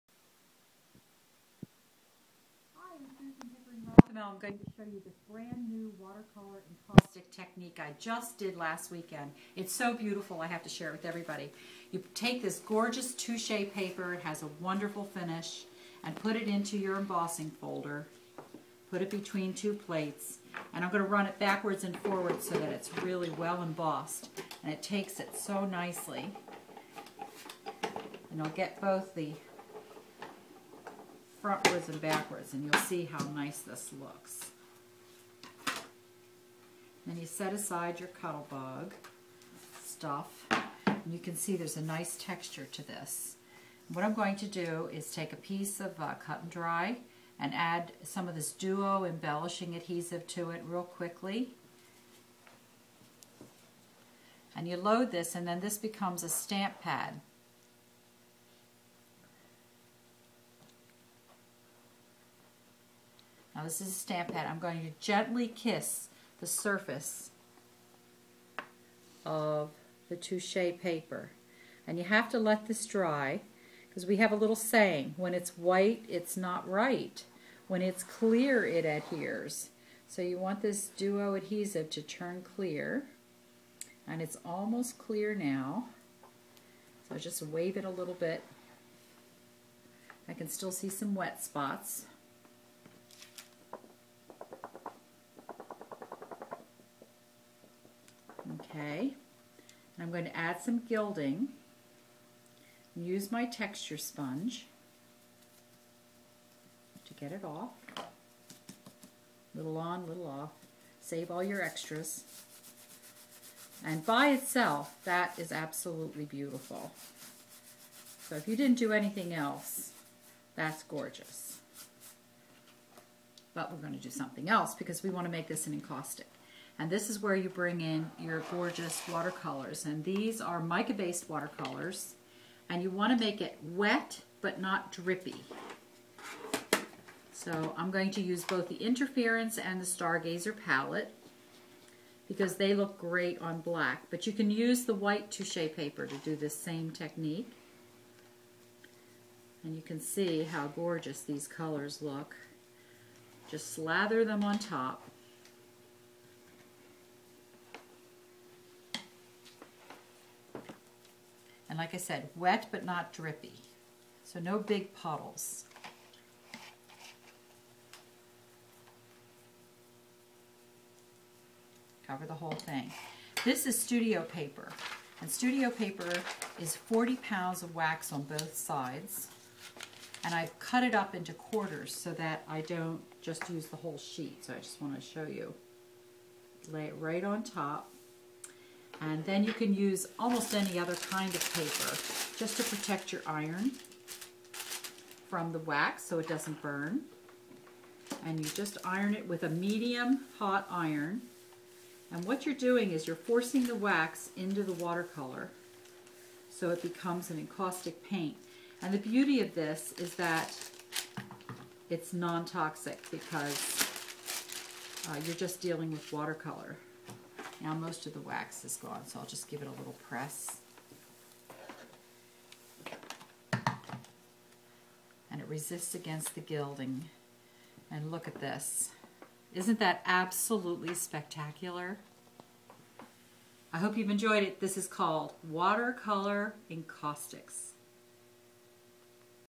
Watercolor Encaustic Part 1
Brand new technique brought to you from Susan Pickering Rothamel at USArtquest. To learn more about these products or to purchase them please visit our